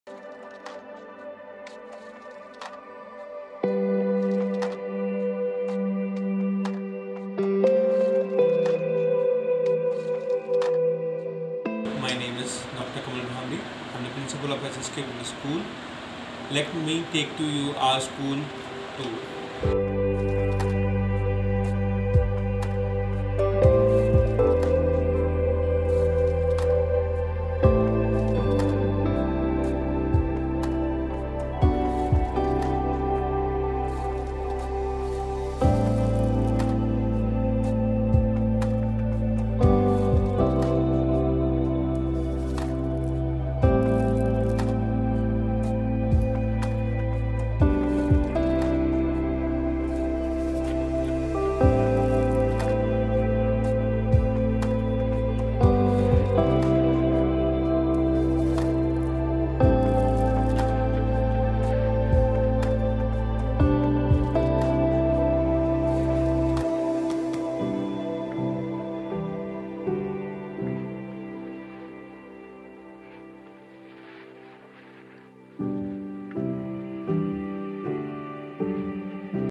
SSK Public school, Ghaziabad, Uttar Pradesh | Virtual School Tour 2022
Located in Ghaziabad, Uttar Pradesh, SSK Public School (CBSE) provides quality education to students from nursery to grade 10.
This LEAD-powered school highlights the importance of a well-rounded education curriculum with its smart classes, art activities, debate and music clubs, libraries, labs, and much more!
Let’s have a quick look around with this virtual tour